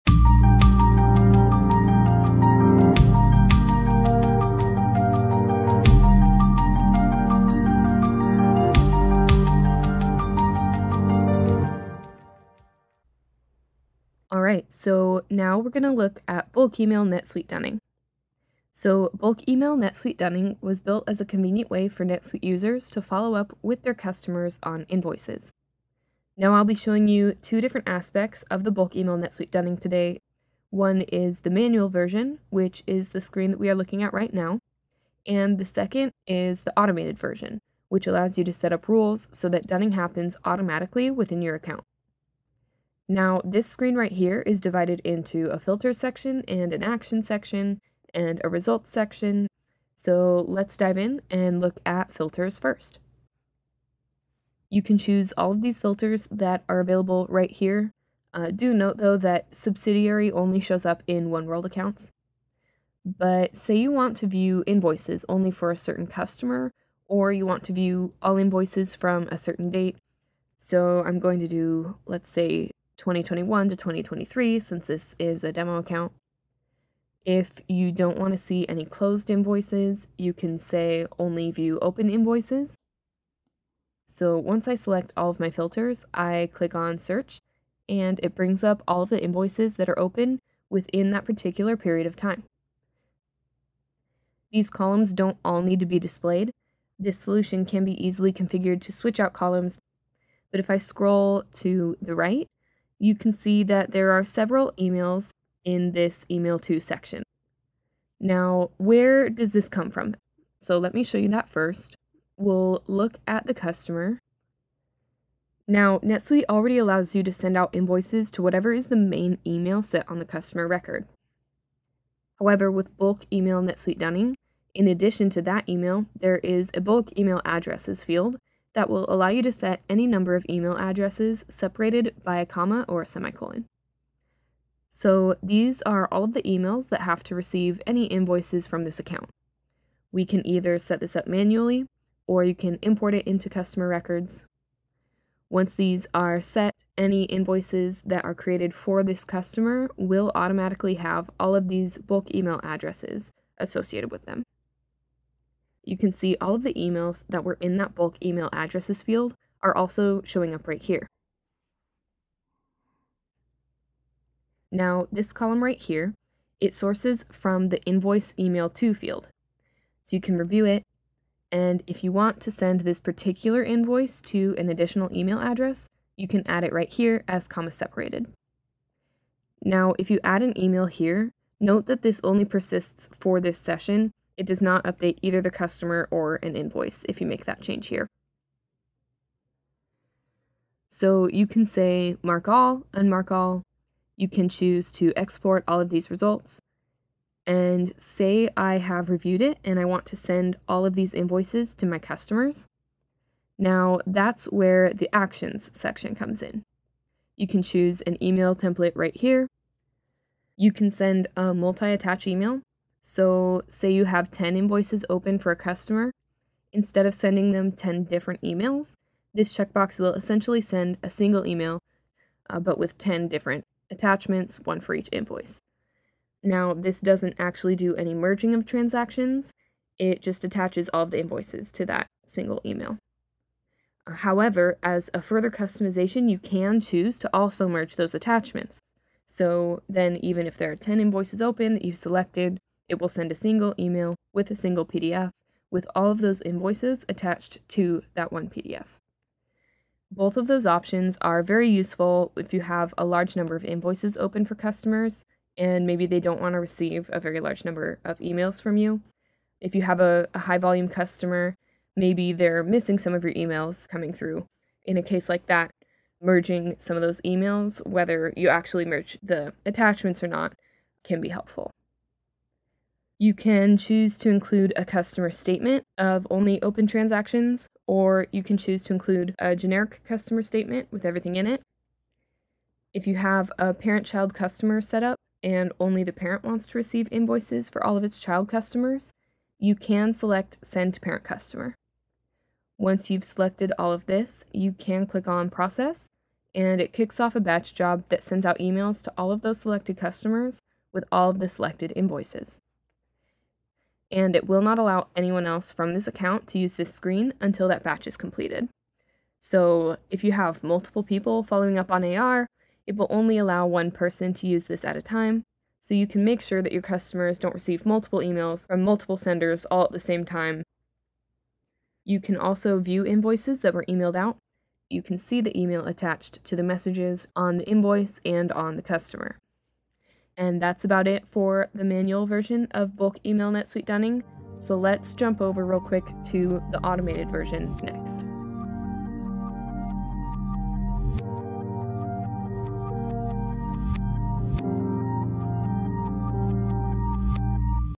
Bulk-email NetSuite Dunning | Demo (Manual Version)
Discover the basic features and capabilities of the manual version of Bulk-email NetSuite Dunning!